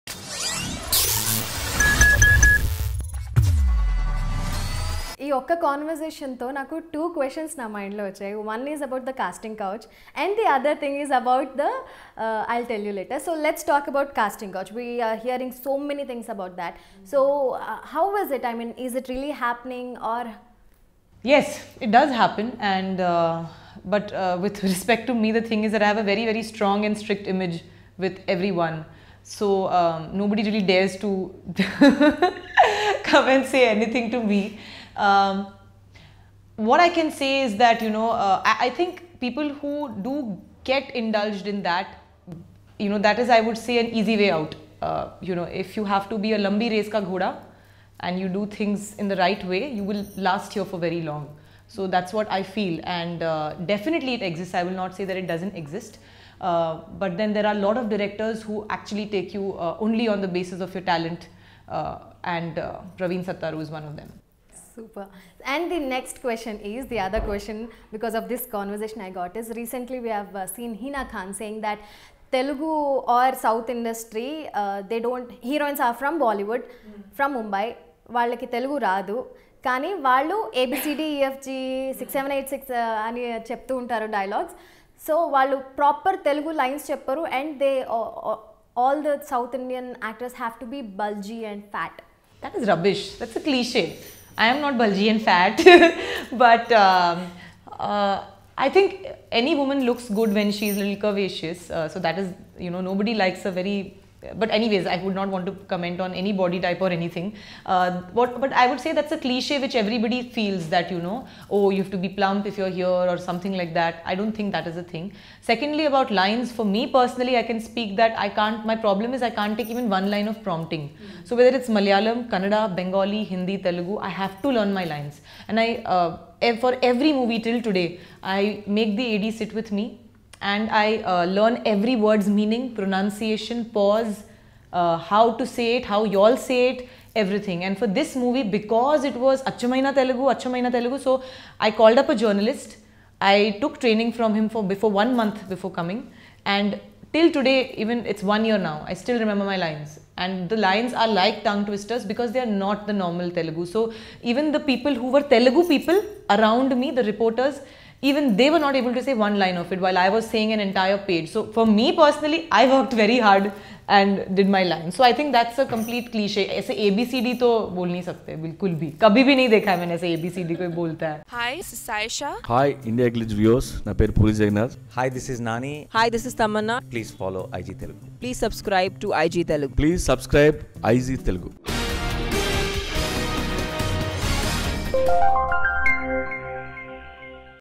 నన్ను వచ్చి ప**మనే దైర్యం లేదు వాళ్లకి | Shraddha Das on casting couch in Tollywood & Bollywood
LetsRewind Shraddha Das talks about casting couch in Tollywood, Bollywood and Hina Khan's comments on South industry. ShraddhaDas recently acted in PSV Garuda Vega. Her other movies include Guntur Talkies, Dictator, Great Grand Masti, Mogudu and more.